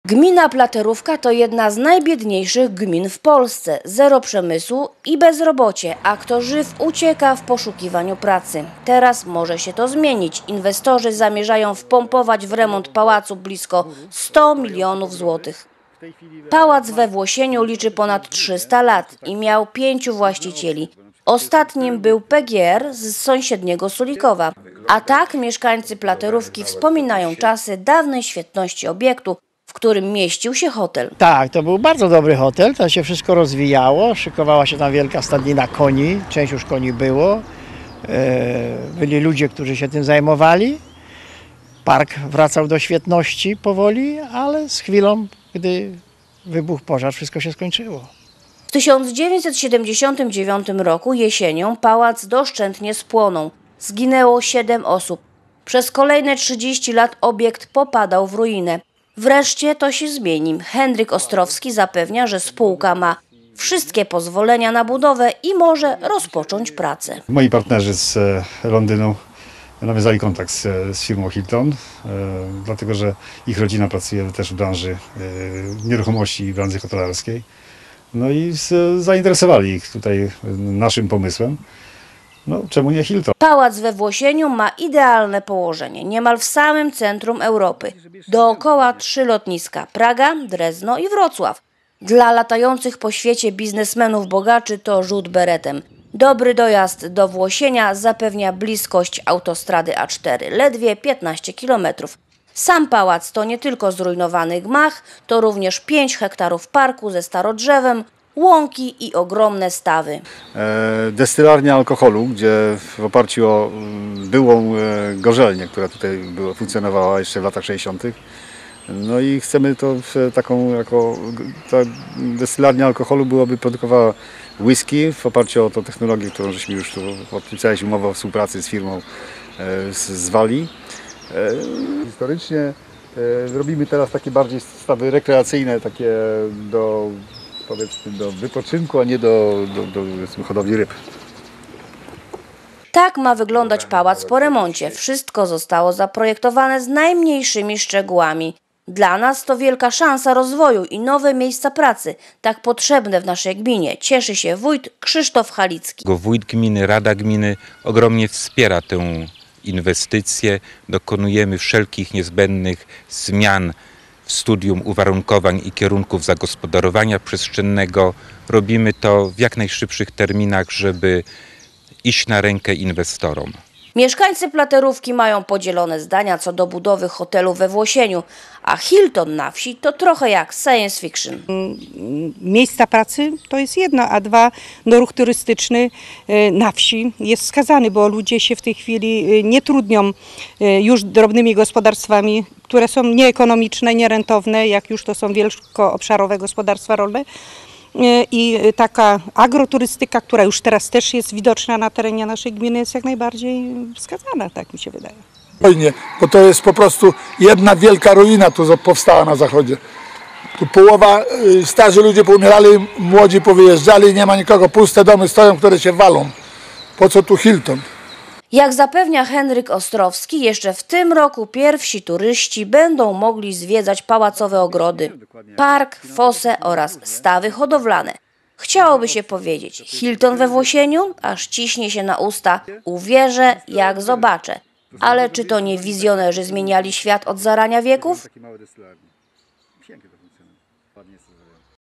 Hilton we Włosieniu
W maleńkiej wsi Włosień w gminie Platerówka, polsko-angielska firma rozpoczęła renowację ruin pałacu. Później powstanie tu pięciogwiazdkowy hotel sieci Hilton razem ze spa, polami golfowymi i destylarnią whiskey.